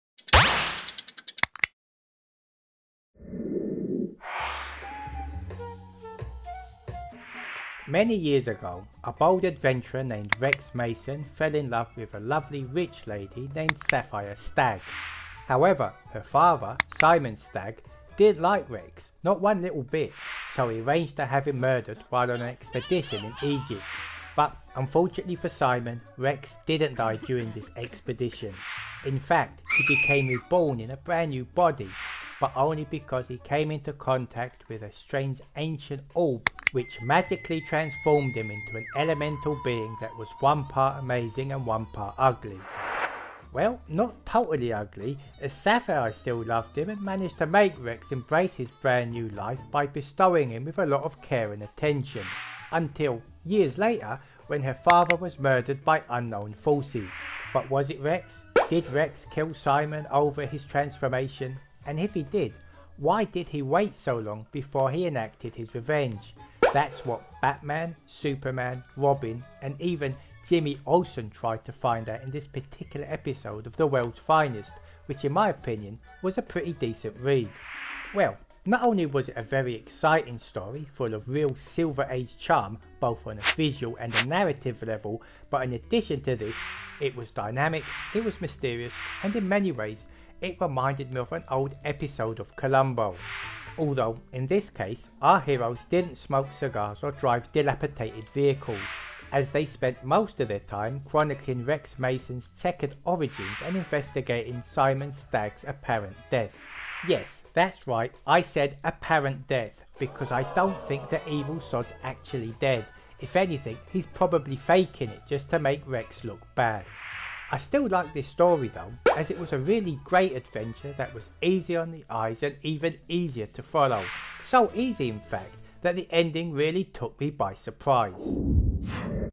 Batman/Superman: World’s Finest #13
In the pages of World’s Finest, Batman, Robin, and Superman have traversed some of the DCU’s most iconic locations and teamed up with the likes of the Teen Titans, Doom Patrol, and Supergirl, but none of that will prepare them as they must go across the globe in search of Rex Mason, a.k.a. Metamorpho, the Element Man! The World’s Finest Strangest Adventure begins here!

Published by: DC Comics
Writer: Mark Waid
Art by: Dan Mora